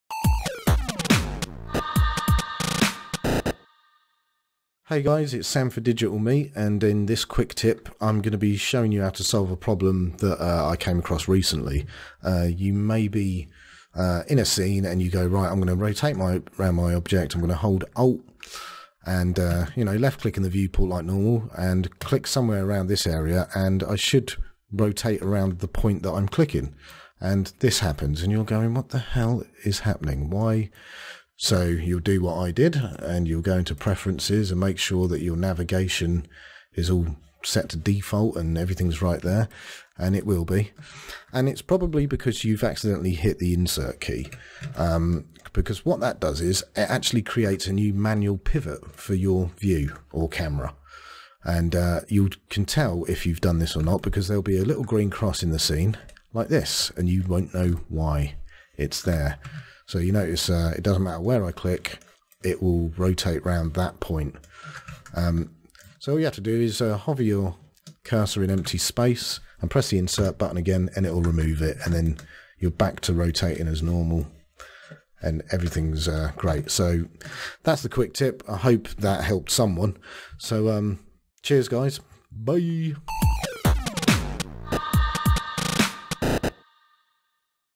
Quick Tip 12: Why Is Viewport Orbiting Messed Up?
In this Cinema 4D quick tip you take a look at one of the reasons your viewport orbiting may not be working as expected.